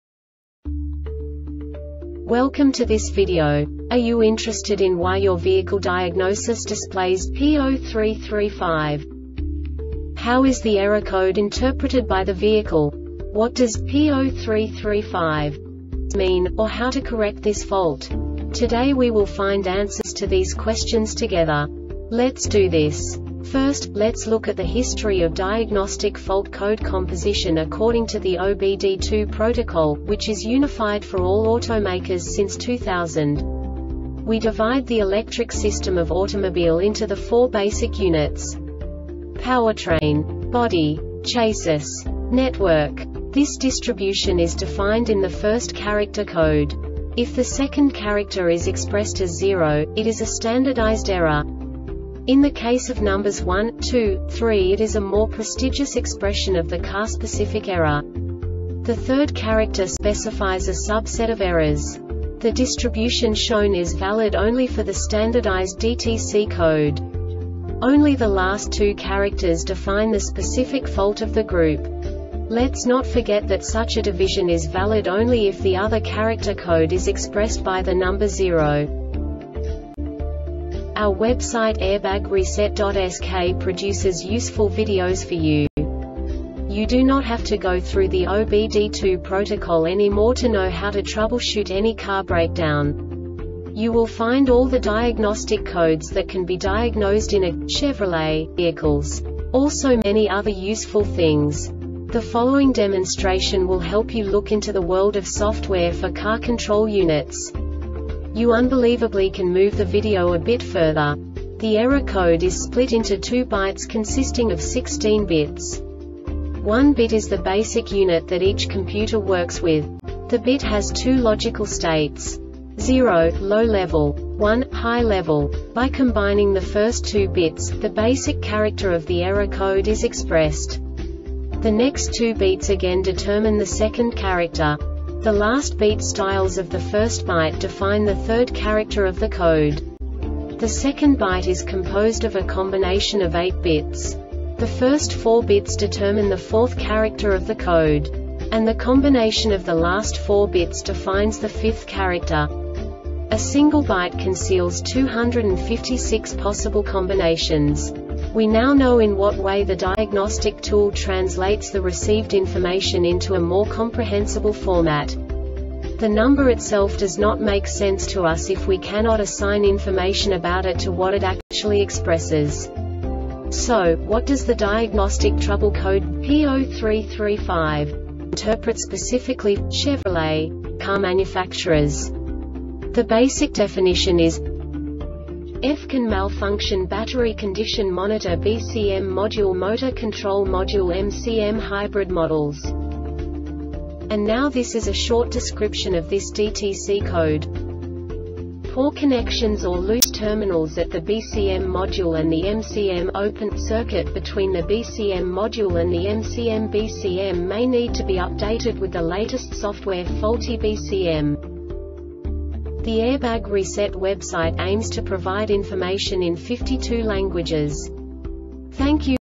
DTC Chevrolet P0335 Short Explanation
Contents:
0:21 Basic DTC analysis according to OBD2 protocol standard.
1:48 Insight into programming
2:58 A diagnostically understandable description of the Diagnostic Trouble Code
3:05 Basic error definition

Make:
Chevrolet
Code:
P0335
Definition:
F-CAN Malfunction (Battery Condition Monitor (BCM) Module- Motor Control Module (MCM) (HYBRID MODELS)
Cause:
Poor connections or loose terminals at the BCM module and the MCM"Open" circuit between the BCM module and the MCMBCM may need to be updated with the latest softwareFaulty BCM